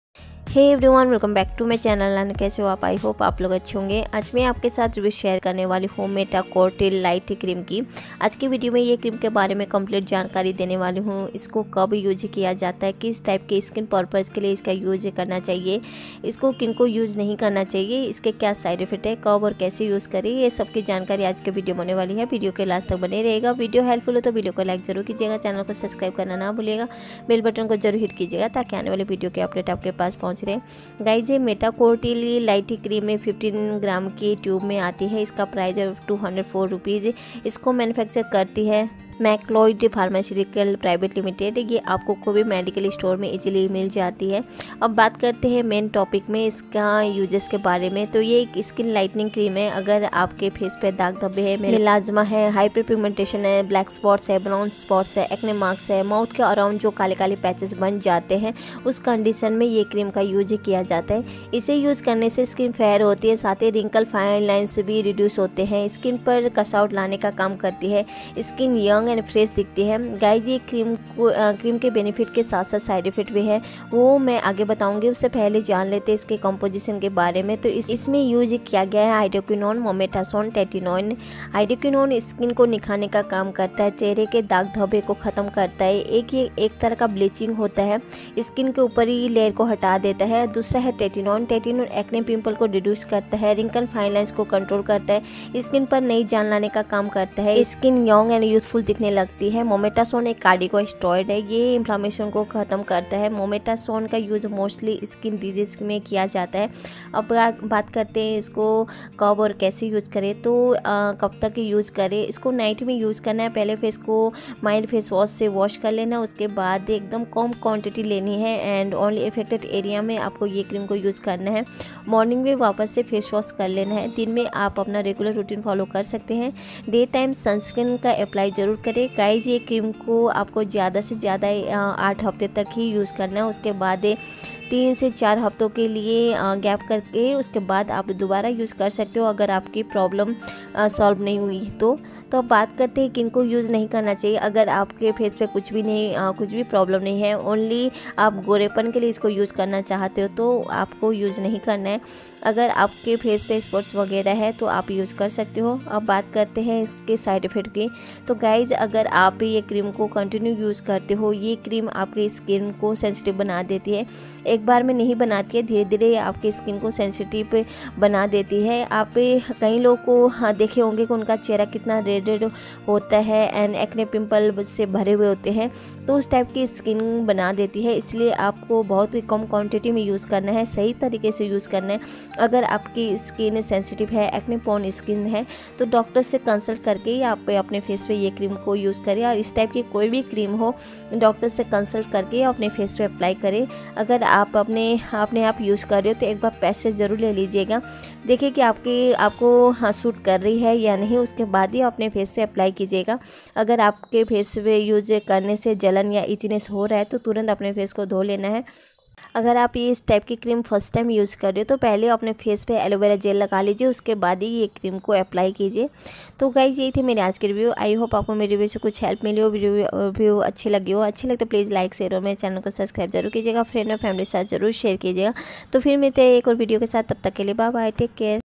Metacortil lite cream use | सब creams से अच्छी क्रीम है। metacortil lite cream side effects
Apollopharmacy :
Snapdeal :
Shopclues :

BENEFITS OF METACORTIL-LITE CREAM
In Treatment of Melasma
Melasma is a common skin problem that causes dark, discolored patches on your skin. It is more commonly seen in females than males. Metacortil-Lite Cream helps to lighten these dark patches of the skin usually caused by pregnancy, birth control pills, hormone medicine, or injury to the skin. It effectively blocks that process in the skin that leads to discoloration. Metacortil-Lite Cream also reduces any redness, rash, pain or itchiness that may be caused in this condition.

This improves your self-esteem and confidence as your appearance changes. You should always use it as prescribed and only apply the amount you have been told to. Keep using it for as long as it is prescribed to get the full benefits.

Top 11 Collection Kurtis from Myntra below 599/-